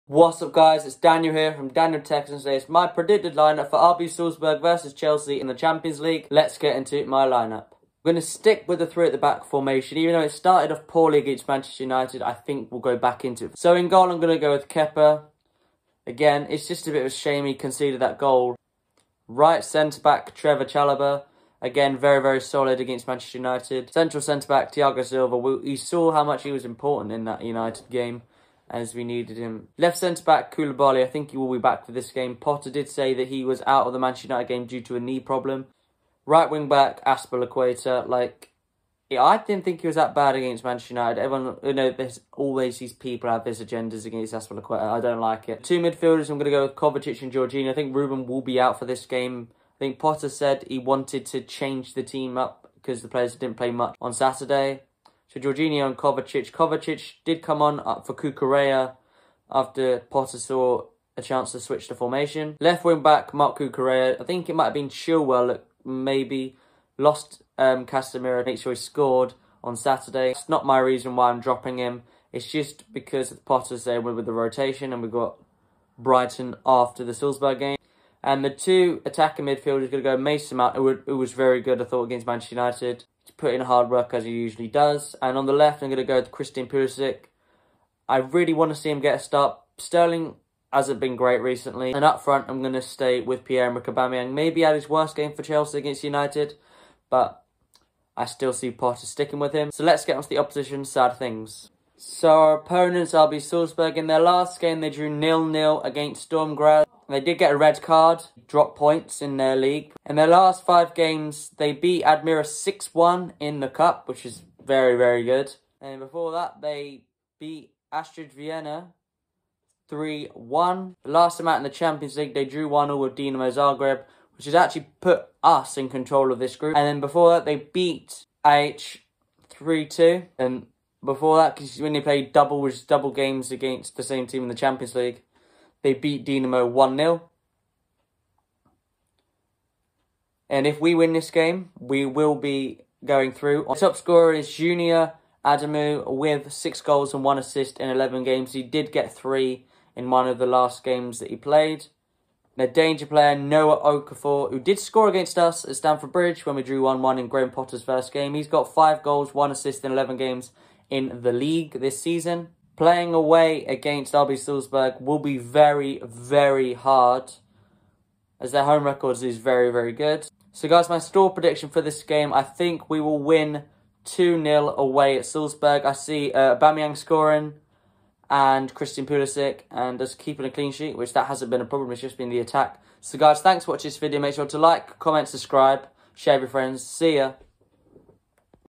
HEAVY ROTATION FROM POTTER? CRUCIAL UCL GAME! | RB SALZBURG VS CHELSEA PREDICTED LINEUP
It’s the return of the champions league! Chelsea face RB Salzburg away in Austria in a huge game in the UCL if we win we will progress to the round of 16 with our two wins against AC Milan have changed the landscape of this group, live stream starting at 17:40 tomorrow!